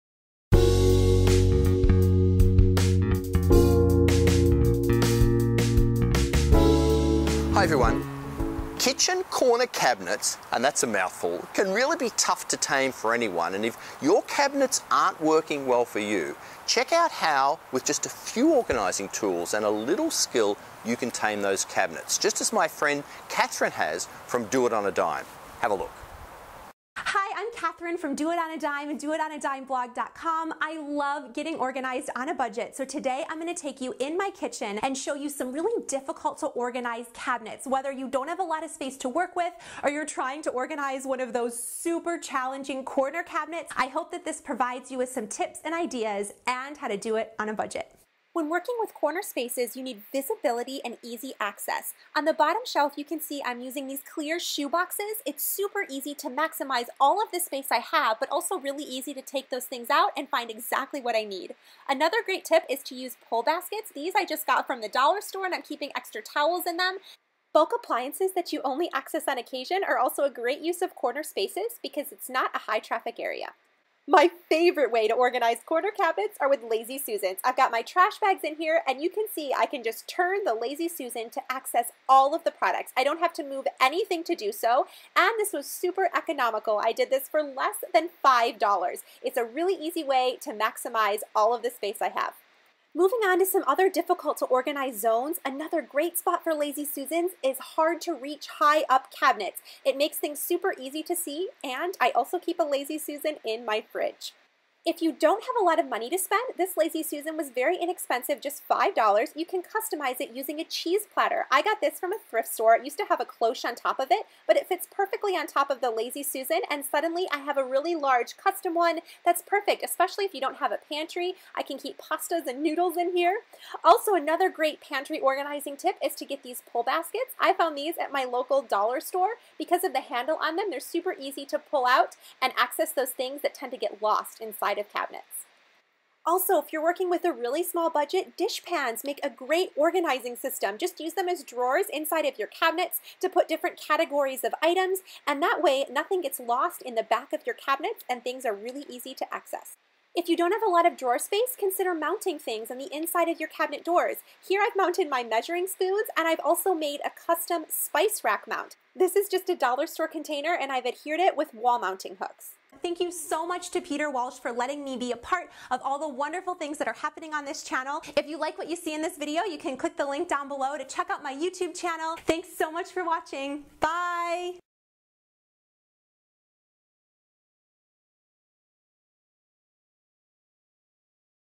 Organizing Kitchen Cabinets | Peter's Organizing Pals
My friend Kathryn from DoItOnADime has some really smart advice for helping you get your kitchen cabinets in order! This is a video jam-packed with great ideas!